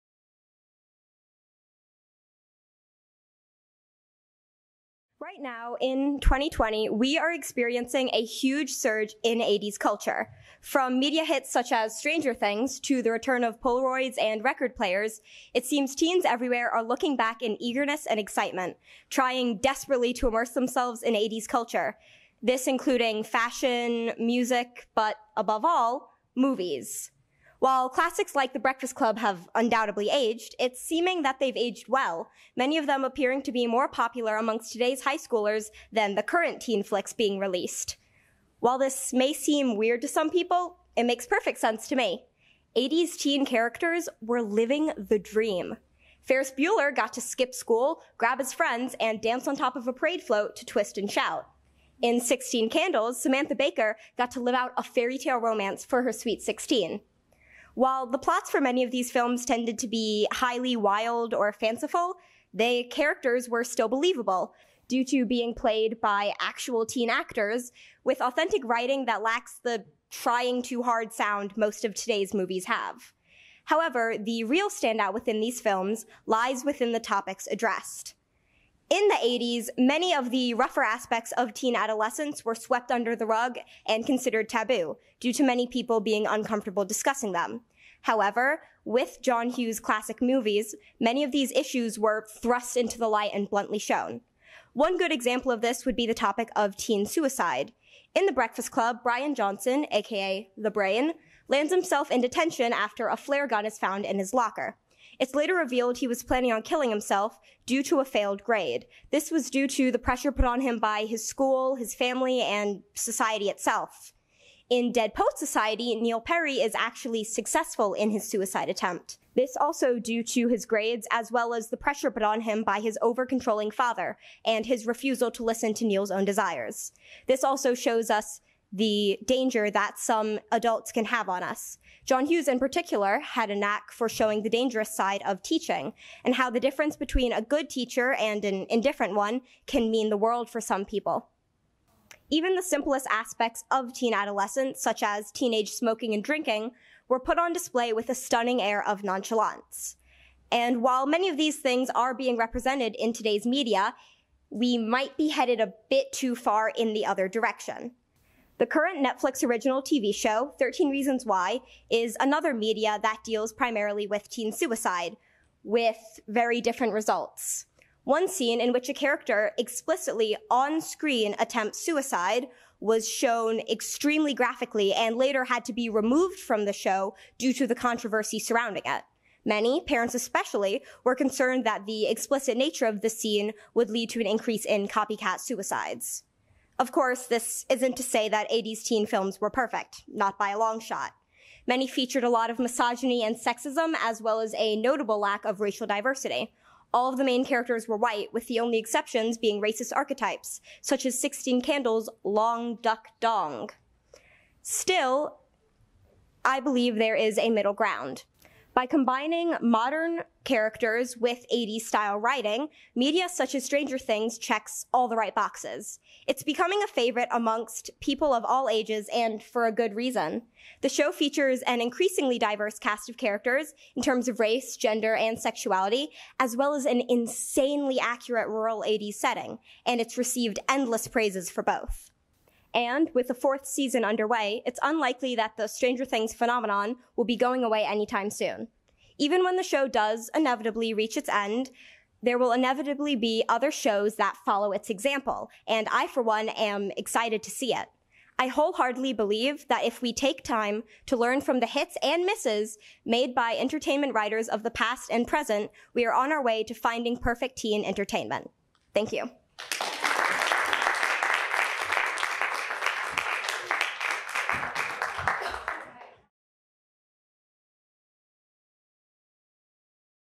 The Teen Experience through 80s Films | Caroline Westphalen | TEDxYouth@OCSA
Caroline Westphalen discusses how John Hughes threw tabooed topics in the faces of his viewers. She compares the teen experience as portrayed in 1980s films to that of today's films. Caroline is a sophomore creative writing major at the Osceola County School for the Arts. This talk was given at a TEDx event using the TED conference format but independently organized by a local community.